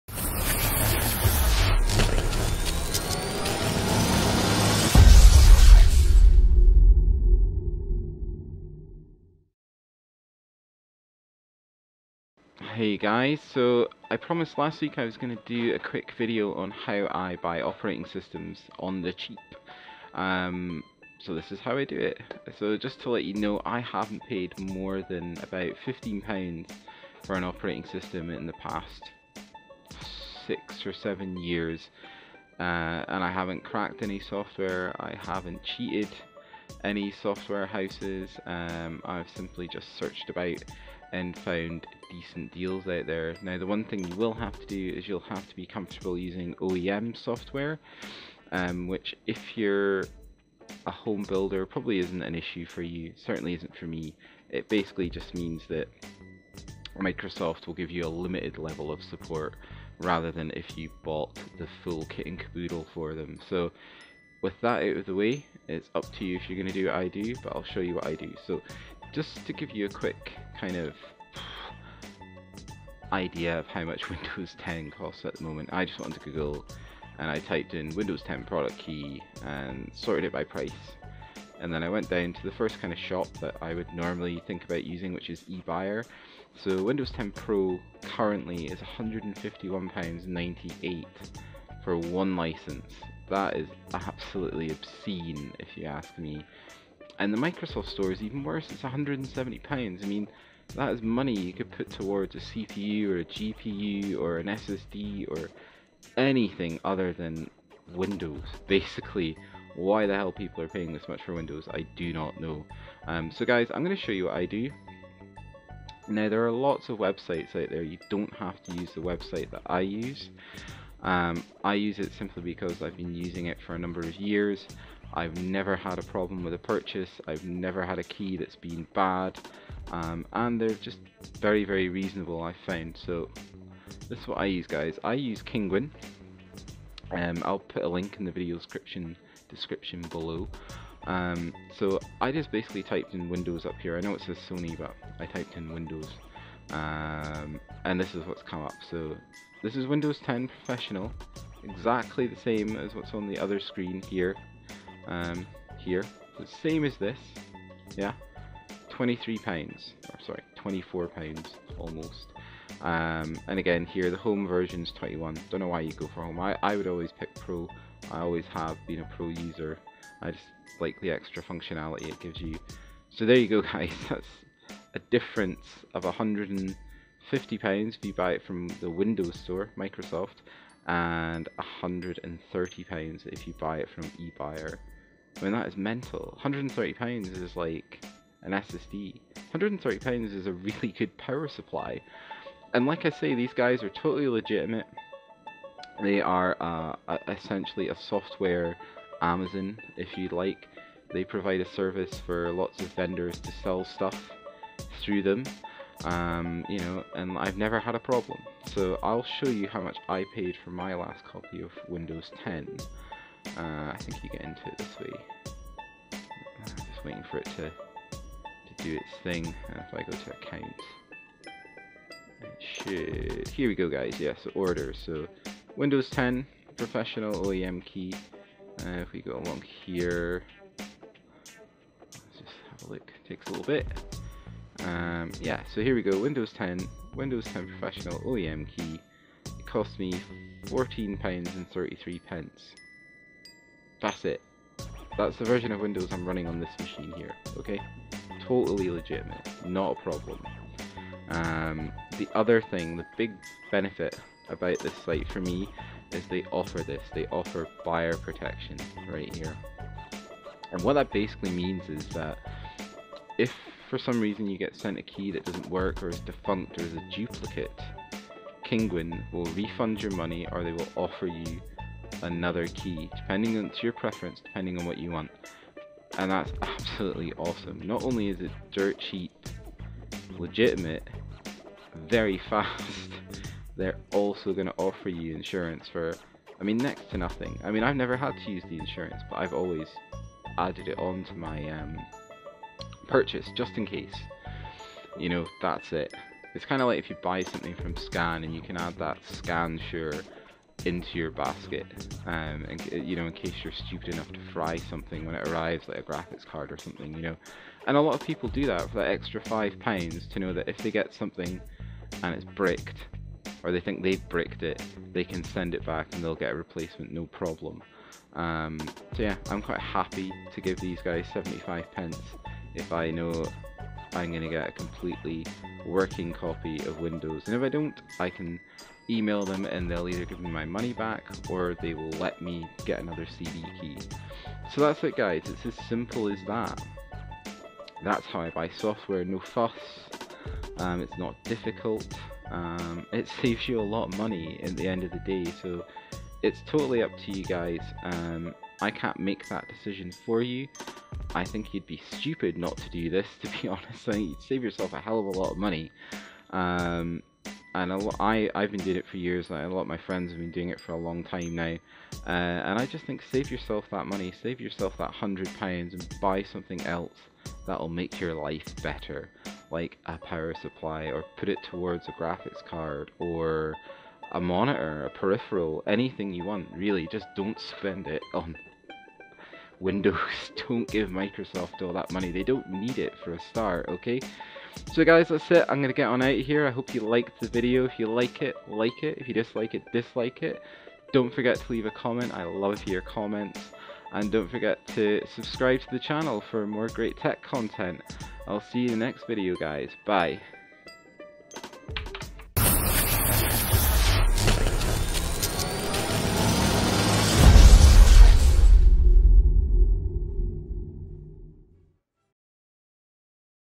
Windows ten Pro for $20!!!!!!!!
Hi Guys,

Ever wished you didn't have to pay the Microsoft tax for a new copy of windows when you build a new machine? Well fear not because I have the answer, watch this video to find out why and how I haven't paid more than 20 pounds for a legitimate copy of windows in years, enjoy!